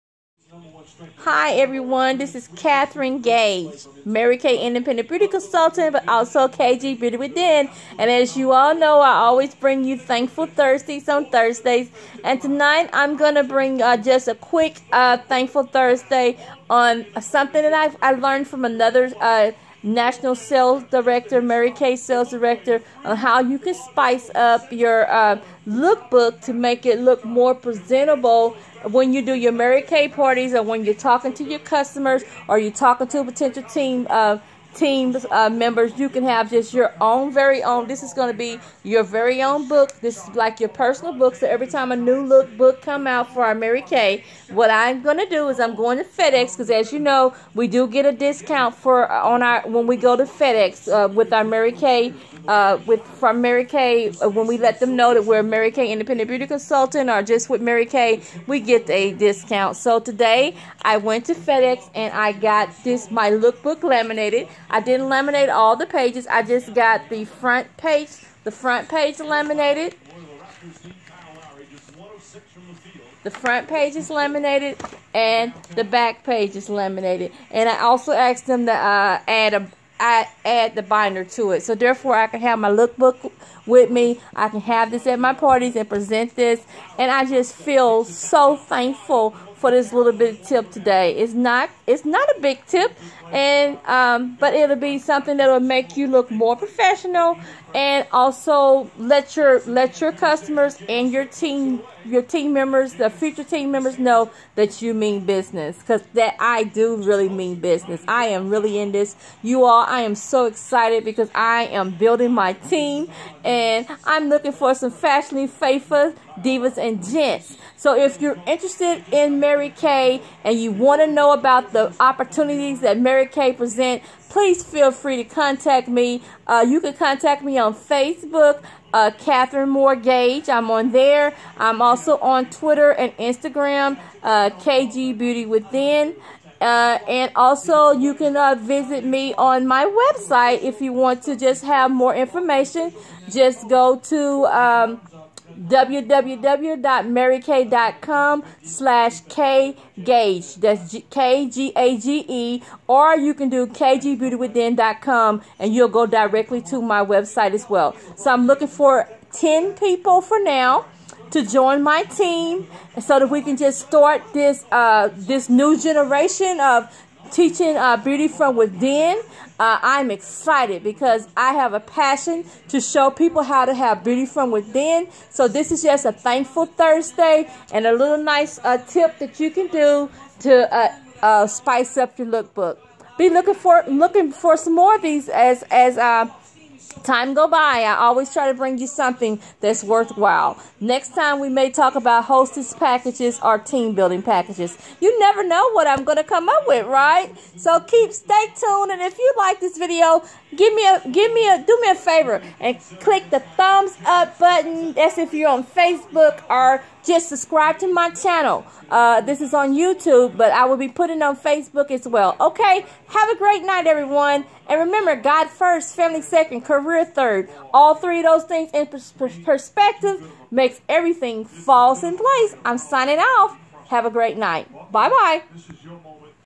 Mary Kay Team Building Tips
How to spice up your Mary Kay Look Book for parties, team building, and display